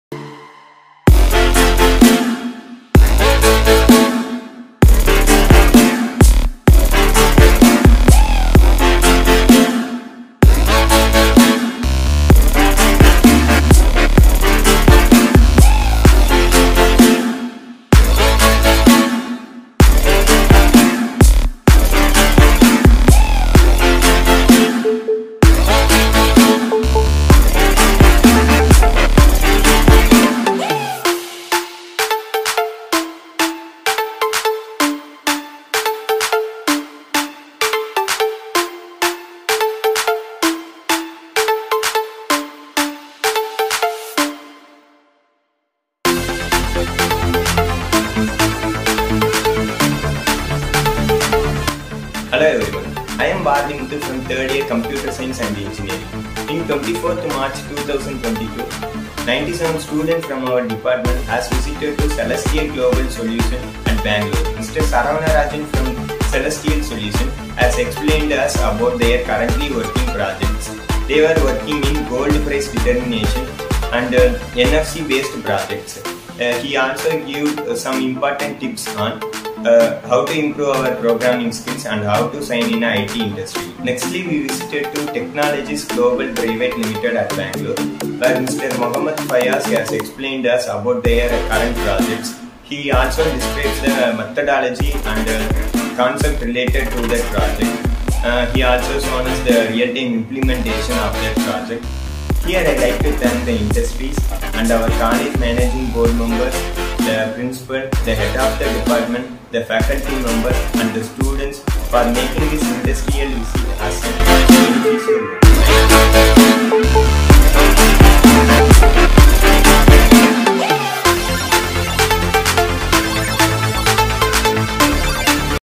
Industrial Visit of III Year - CSE A & B
Two days from 24/03/2022 to 25/03/2022 industrial visit to Bengaluru
(Celestial V Solutions Company and Visvesvaraya Industrial & Technological Museum).